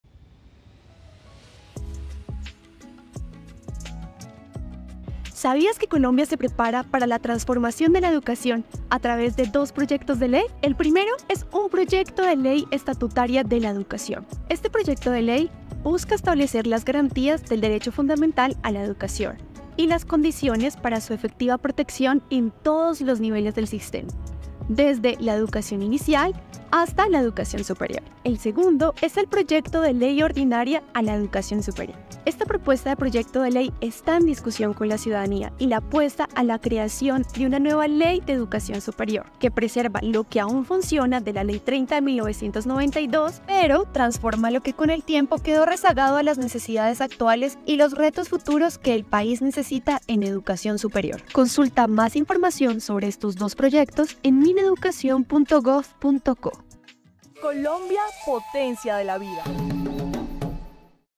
Proyectos que transformarán la educación en Colombia
El proyecto de ley estatutaria y la reforma a la Ley 30 transformarán la educación en Colombia. Conócelos y haz parte del cambio.